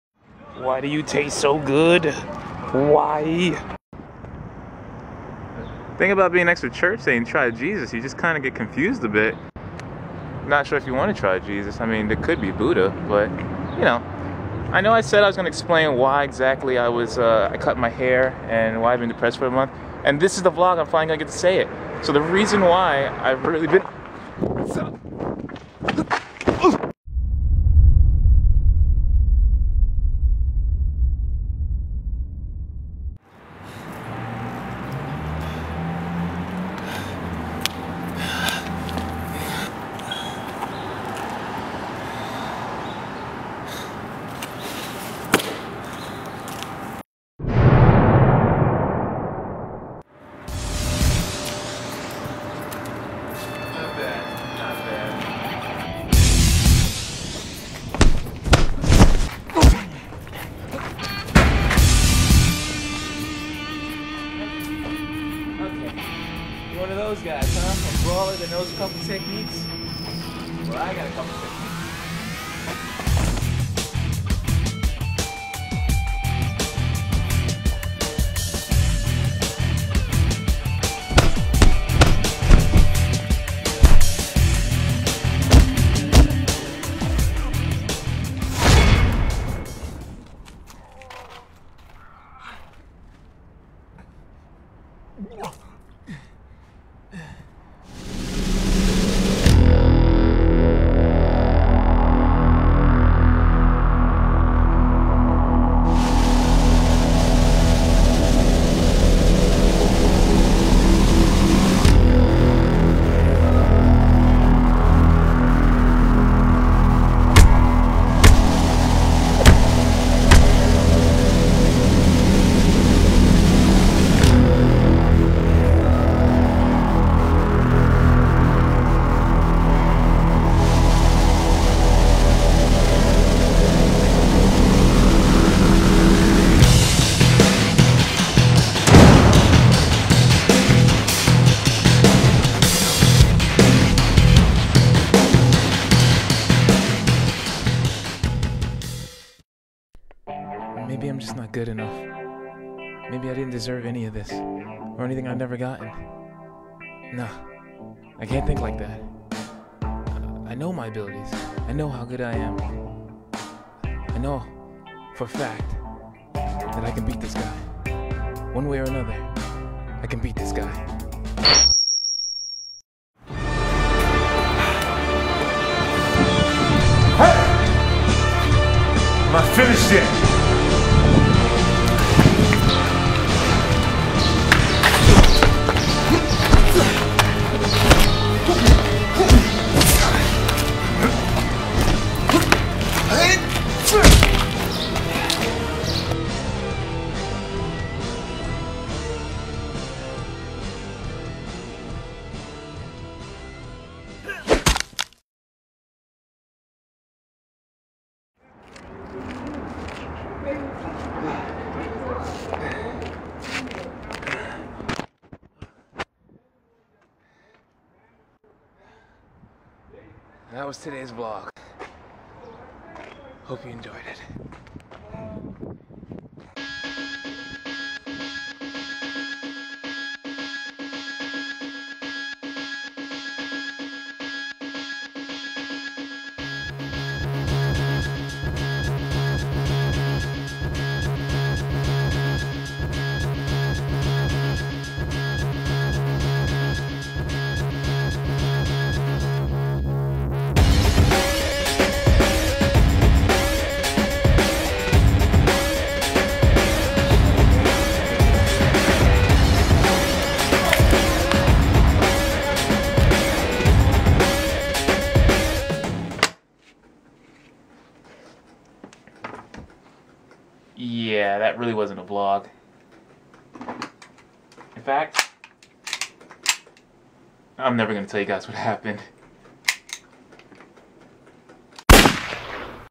Attack The VLOG!
So finally, after almost 2 months I finally explain why I cut my hair, where I was for all that time, an why I was so depressed. or do I? BTS COMING SOON!